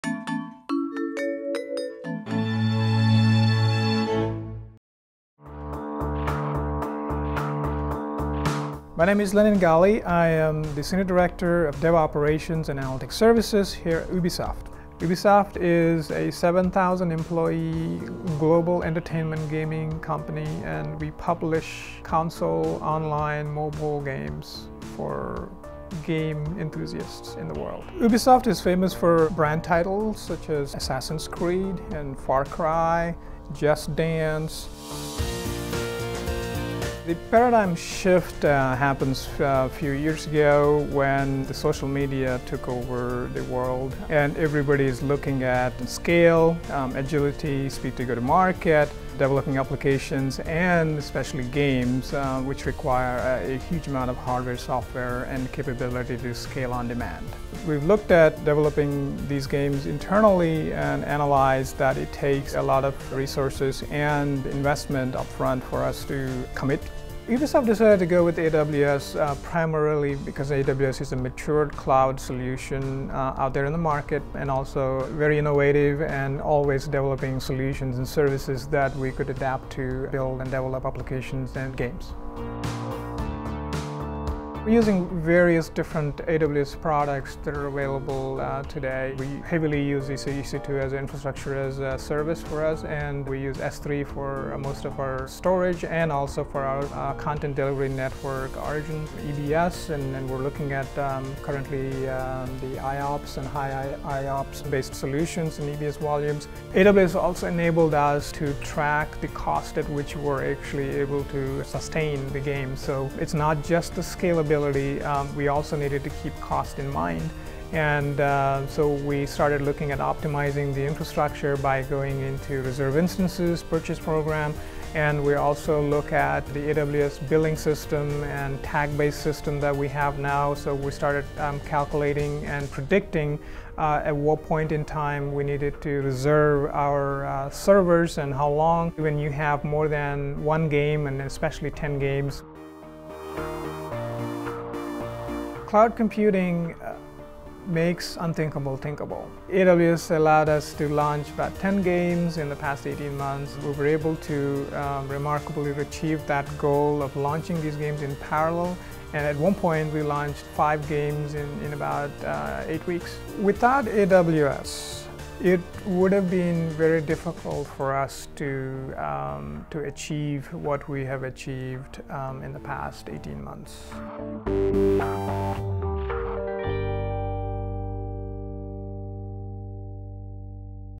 Ubisoft on AWS - Customer Success Story
Learn how one of the largest gaming companies in the world, Ubisoft, leveraged the flexibility of Amazon Web Services to successfully launch 10 games in 18 months.  Ubisoft selected AWS due to its broad set of services that made building, deploying, and scaling their games easier. Click here to find out more about building games on the AWS Cloud